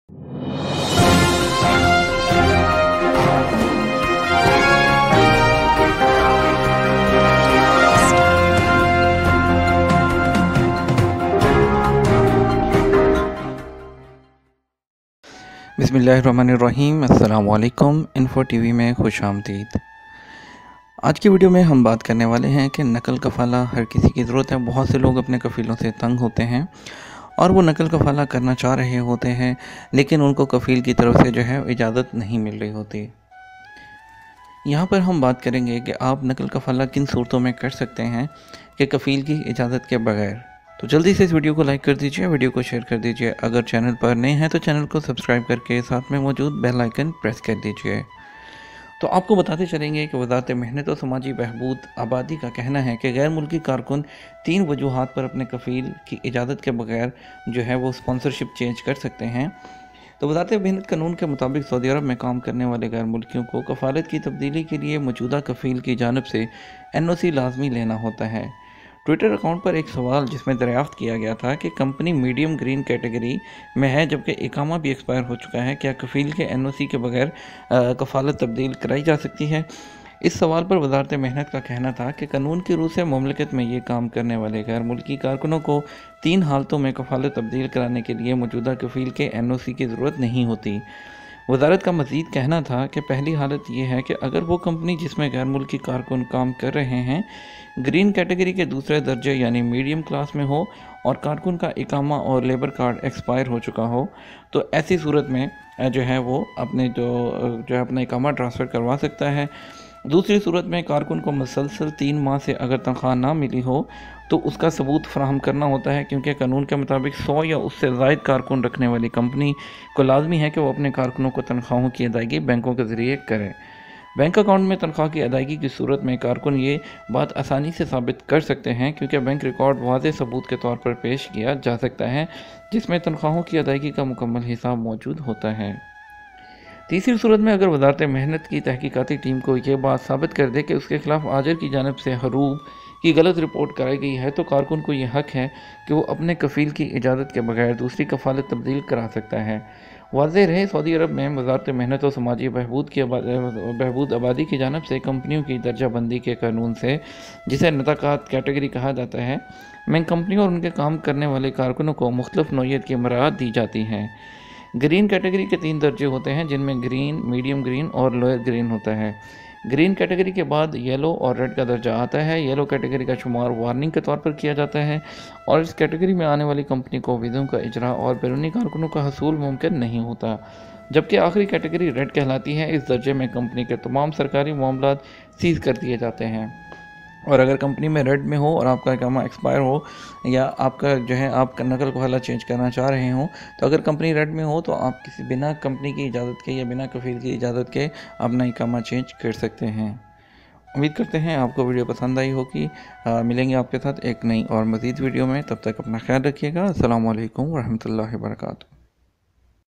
Saudi Arabia Latest Update about Sponsership || کفالت کی تبدیلی کفیل کی اجازت ضروری نہیں
وزارت محنت و سماجی بہبود آبادی کا کہنا ہے کہ غیر ملکی کارکن تین وجوہات پر اپنے کفیل (سپانسر) کی اجازت کے بغیر کفالت (سپانسر شپ) تبدیل کرانے کا حق رکھتے ہیں۔
واضح رہے وزارت محنت کے قانون کے مطابق سعودی عرب میں کام کرنے والے غیر ملکیوں کو کفالت کی تبدیلی کے لیے موجودہ کفیل کی جانب سے این او سی لینا لازمی ہوتا ہے۔
ٹوئٹر اکاؤنٹ پر ایک سوال جس میں دریافت کیا گیا تھا کہ کمپنی میڈیم گرین کیٹگری (نطاق اخصر متوسط) میں ہے جبکہ اقامہ بھی ایکسپائر ہو چکا ہے کیا کفیل کے این او سی کے بغیر کفالت تبدیل کرائی جاسکتی ہے ؟
اس سوال پر وزارت محنت کا کہنا تھا کہ قانون کی رو سے مملکت میں کام کرنے والے غیر ملکی کارکنوں کو تین حالتوں میں کفالت تبدیل کرانے کے لیے موجودہ کفیل کے این او سی کی ضرورت نہیں ہو گی۔
وزارت کا مزید کہنا تھا کہ پہلی حالت یہ ہے ’اگر وہ کمپنی جس میں غیر ملکی کارکن کام کررہے ہیں گرین کیٹگری کے دوسرے درجے یعنی میڈیم کلاس میں ہو اور کارکن کا اقامہ اور لیبر کارڈ ایکسپائر ہو چکا ہو۔‘